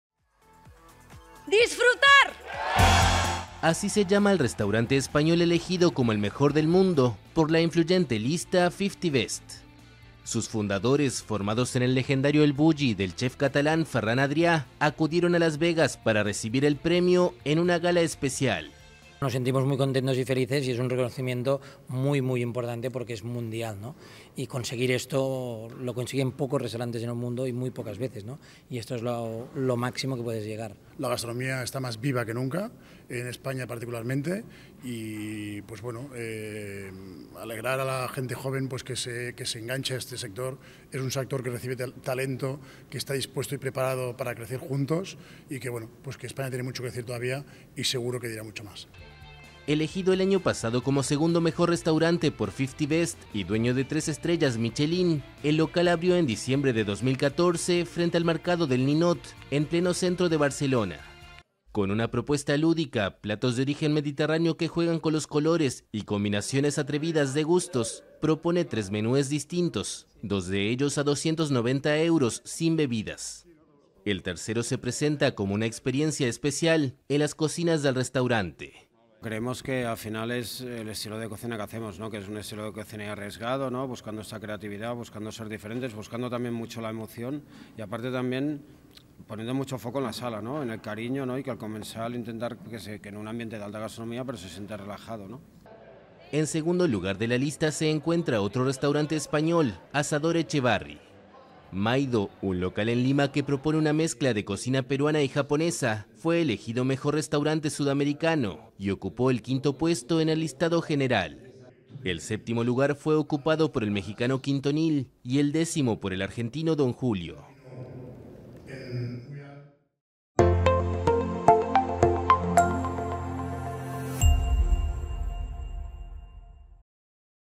El restaurante español Disfrutar es el mejor del mundo, según la lista 50 Best
El restaurante español Disfrutar, situado en Barcelona, fue elegido el mejor del mundo por la influyente 50 Best, de la revista británica Restaurant.